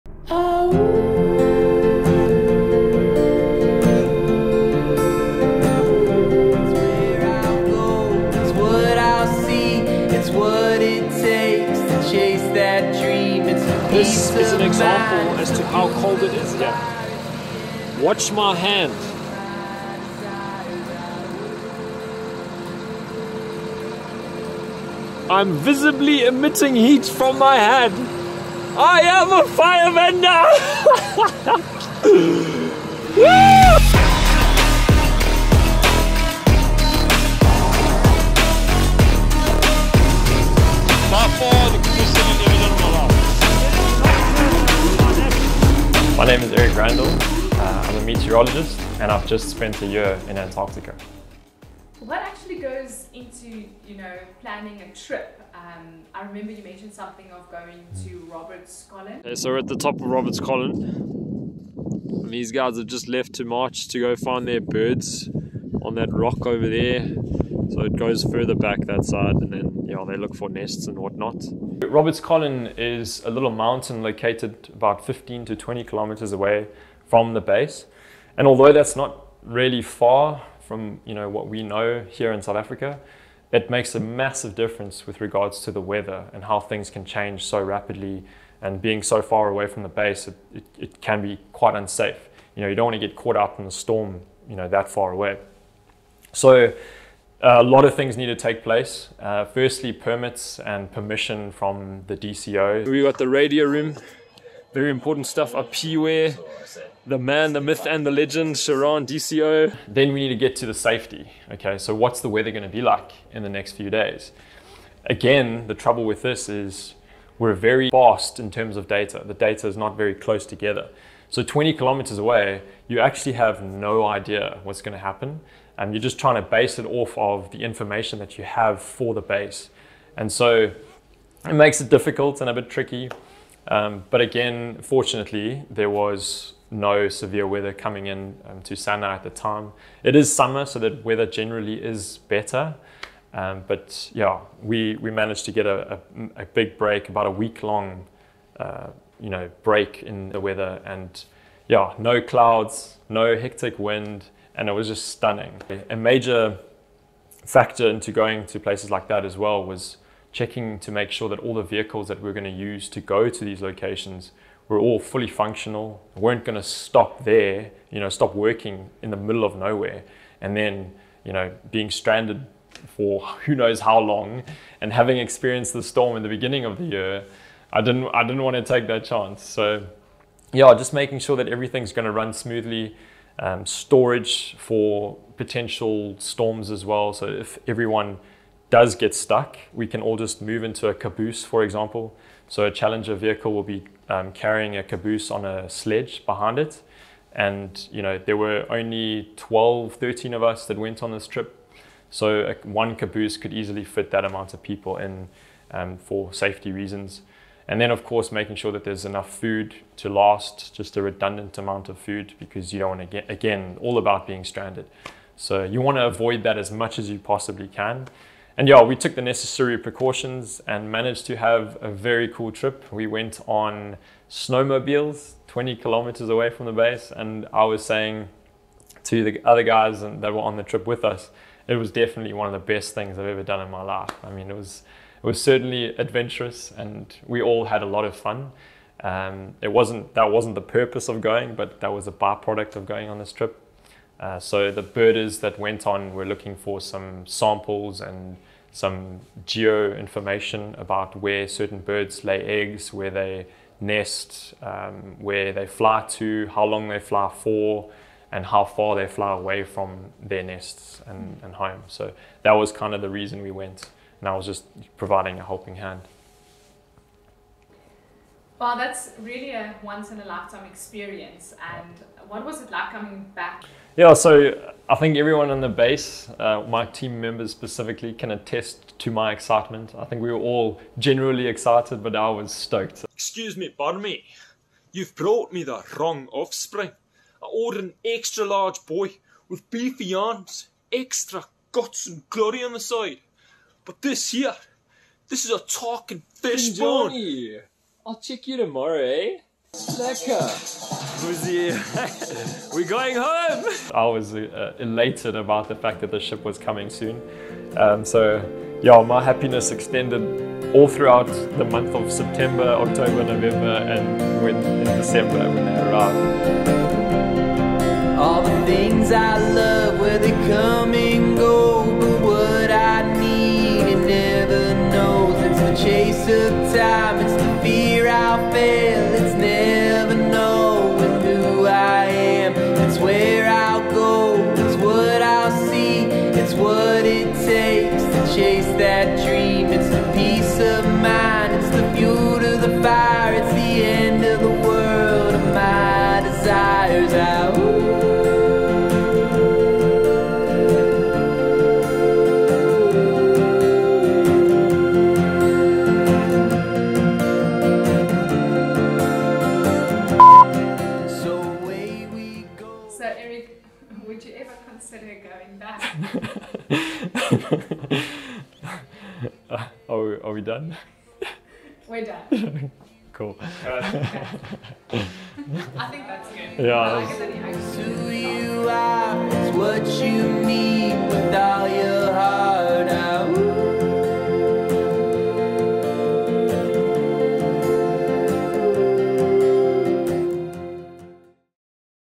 𝗜 𝗦𝗣𝗘𝗡𝗧 𝗔 𝗬𝗘𝗔𝗥 𝗜𝗡 𝗔𝗡𝗧𝗔𝗥𝗖𝗧𝗜𝗖𝗔🔹𝐏𝐀𝐑𝐓 𝟑
Featuring: Meteorologist Eric Randall

This is the 3rd and final part of our short documentary with Meteorologist Eric Randall who spent a year in ANTARCTICA doing research and weather forecasting at SANAE IV
(𝑆𝑜𝑢𝑡ℎ 𝐴𝑓𝑟𝑖𝑐𝑎𝑛 𝐴𝑛𝑡𝑎𝑟𝑐𝑡𝑖𝑐 𝑟𝑒𝑠𝑒𝑎𝑟𝑐ℎ 𝑏𝑎𝑠𝑒 𝑙𝑜𝑐𝑎𝑡𝑒𝑑 𝑖𝑛 𝑉𝑒𝑠𝑙𝑒𝑠𝑘𝑎𝑟𝑣𝑒𝑡, 𝑄𝑢𝑒𝑒𝑛 𝑀𝑎𝑢𝑑 𝐿𝑎𝑛𝑑)

Hope you enjoyed the mini documentary as much as we did ❄️❄️❄️

In case you missed the first two parts of our ii documentary, see below links to watch